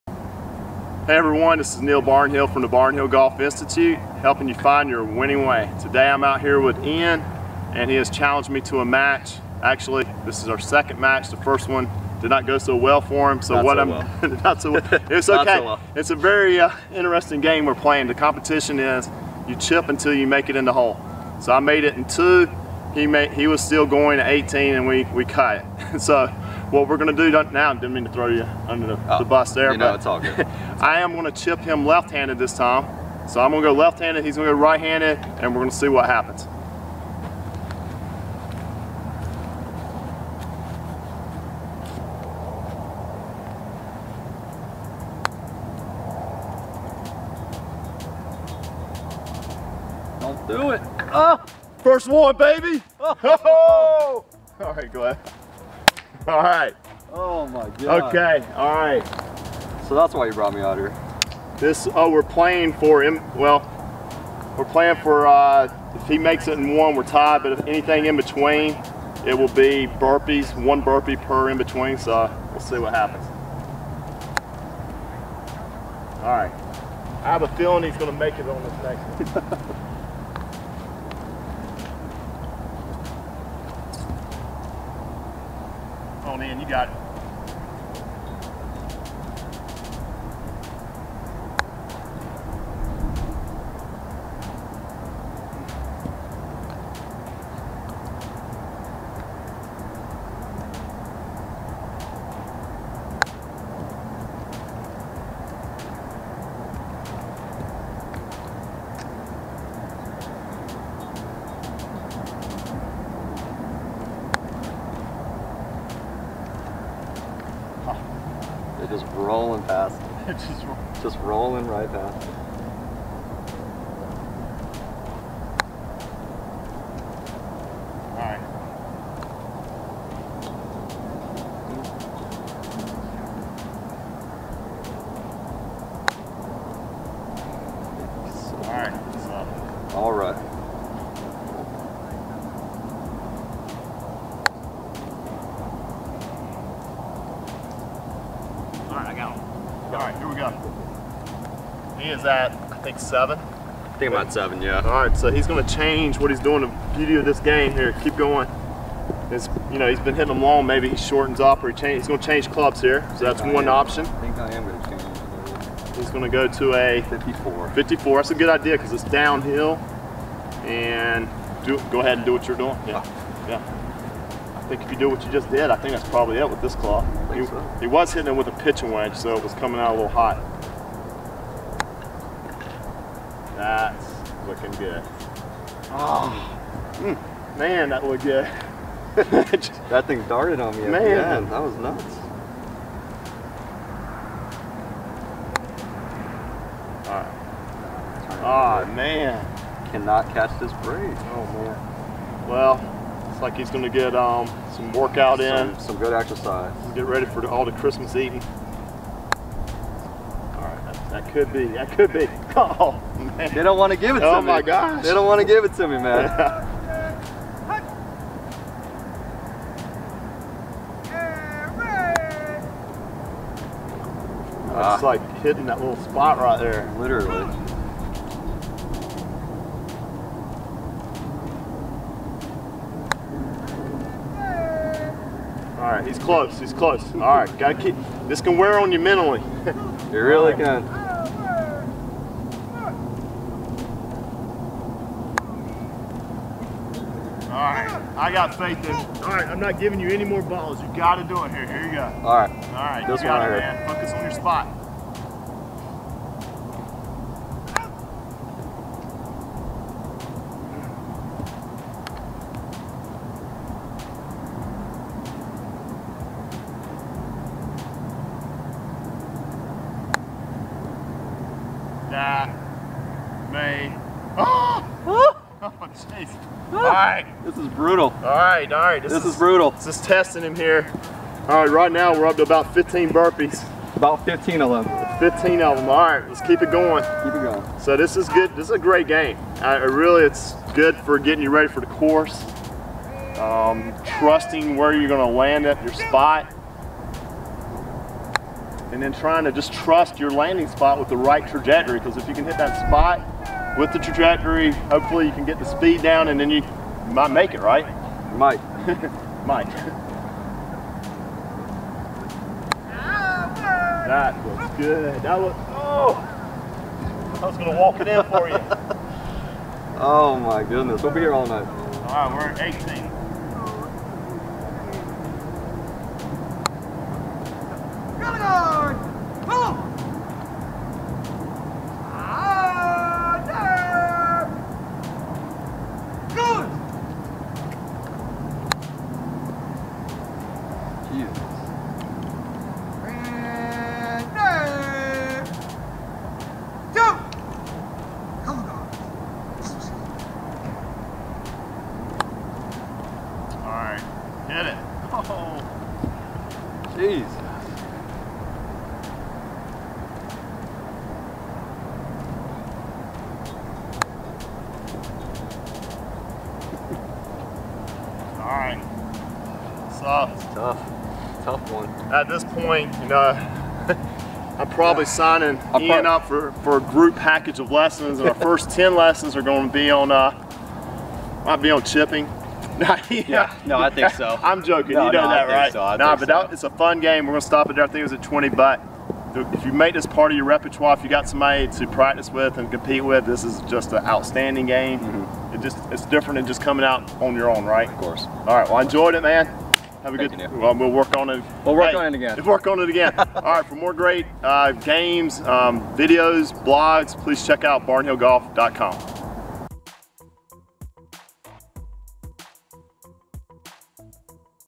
BGI Golf Games: A Chipping Challenge that will get you in shape!
Everyone loves to have fun on the course, or when they practice and there is no better way to do that than to challenge a buddy to a friendly chipping game.  Watch Neil Barnhill of BGI take on one of his buddies with burpees at stake!